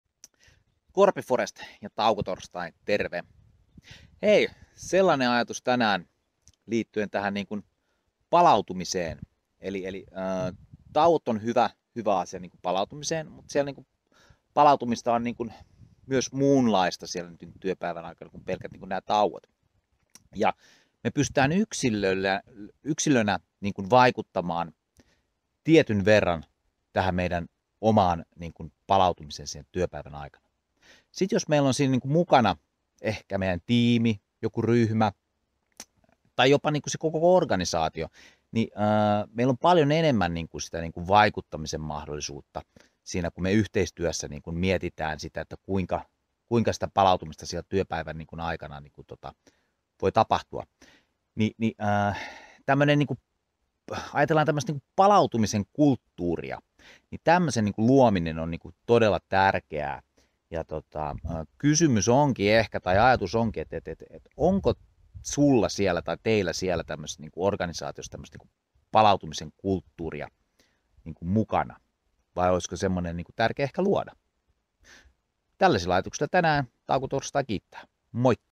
Taukotorstai palautumisenkulttuuri
taukotorstai ajatuksia

Nykyään puhutaan paljon siitä, että palautumista olisi hyvä tapahtua työpäivän aikana, jotta voisimme hyvin ja tätä tutkimukset hyvin tukevat.

Kaikki lähtee yksilöstä, mutta suurena apuna on tiimi, yhteisö, organisaatio.

Yhdessä pystymme luomaan palautumisenkulttuurin, joka isommin tukee meitä jaksamiseen ja kokonaisvaltaiseen hyvinvointiin.

Onko teillä työssä palautumisenkulttuuri?